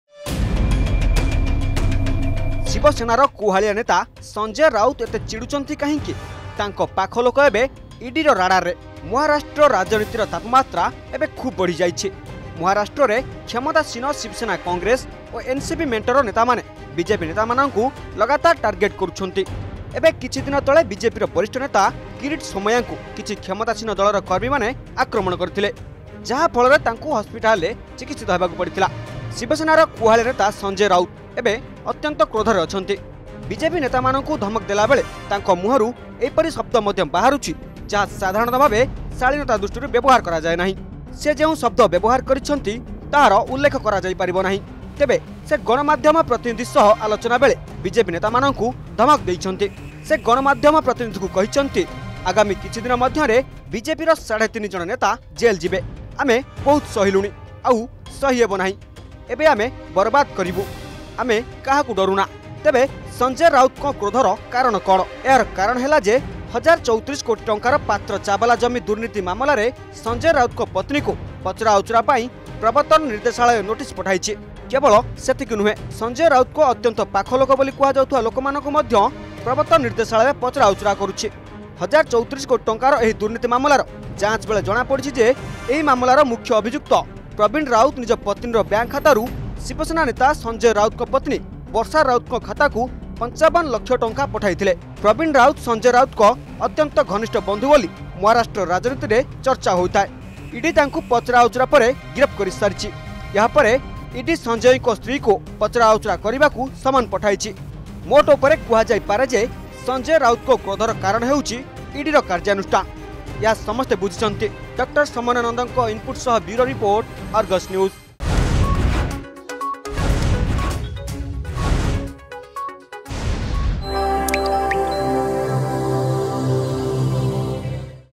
Shiv Sena Leader Sanjay Rout threats BJP Leaders of Putting them Behind bars
Shiv Sena Leader Sanjay Raut claimed that "three and a half" leaders of BJP will be behind bars in the next few days.